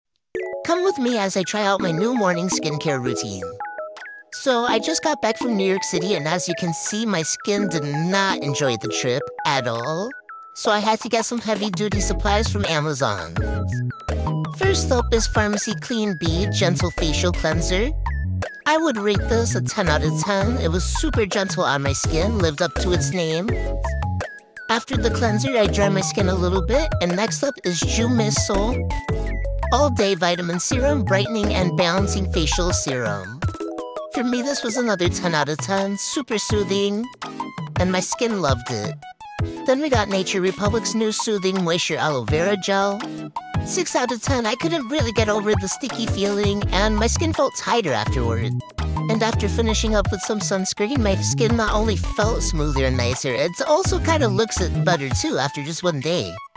Vlog trying out a new skin care routine
First skin care video! Im going to more cons and film festivals so gremlin days are over 🙈🙈🙈